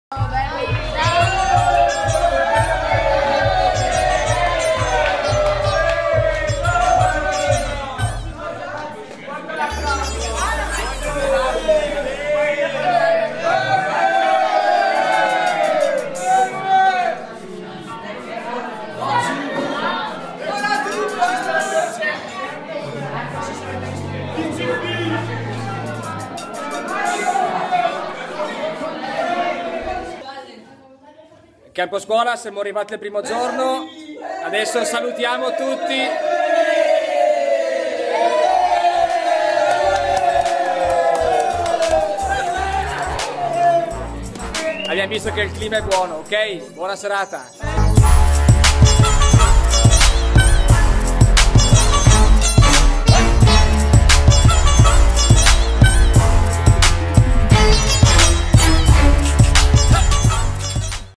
CAMPOSCUOLA TONALE DAY ONE - ORATube
Il primo vlog live dal camposcuola invernale (Tonale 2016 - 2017)